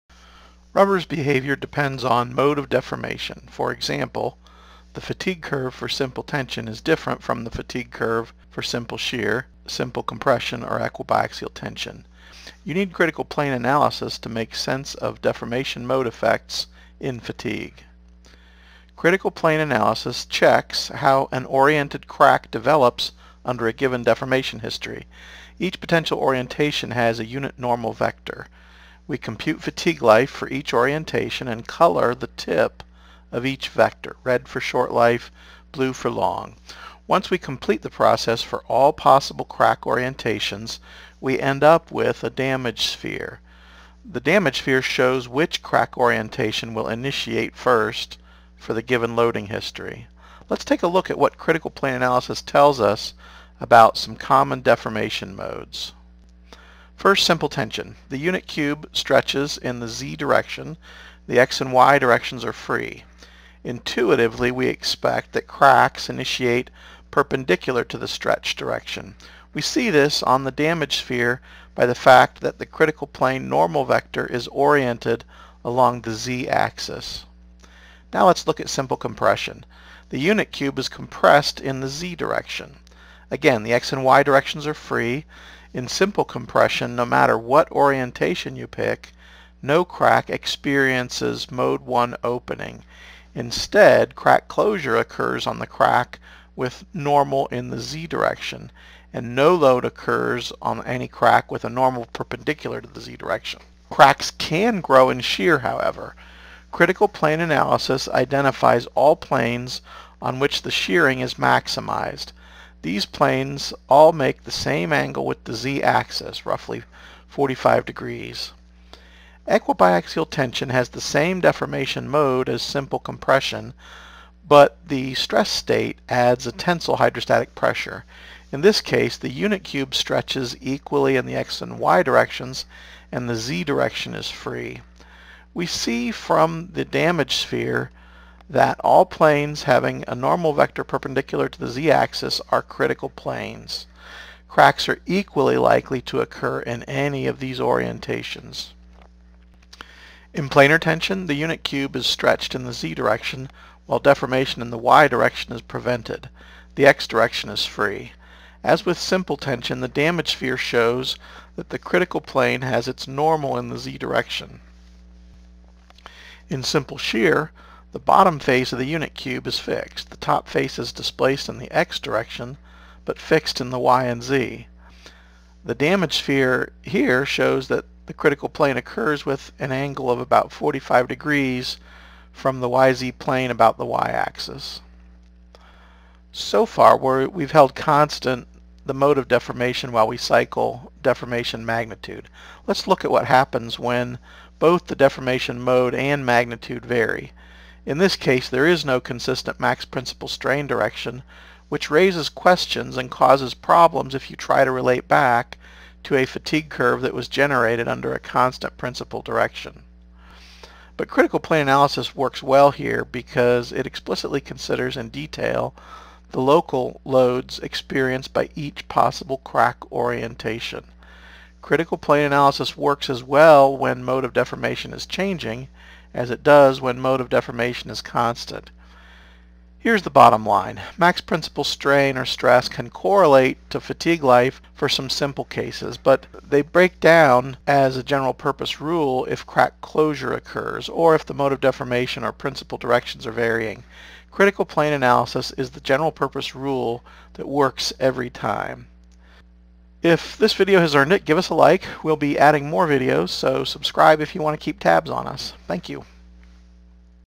Critical Plane Analysis of Common Deformation Modes in Rubber
Shows the failure modes predicted by critical plane analysis for common deformation modes used in testing of elastomers. The damage sphere visualizations were made with the Endurica damage sphere viewer.

Read More about Critical Plane Analysis:
Barbash, Kevin P., and William V. Mars. Critical Plane Analysis of Rubber Bushing Durability under Road Loads. No. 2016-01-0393. SAE Technical Paper, 2016.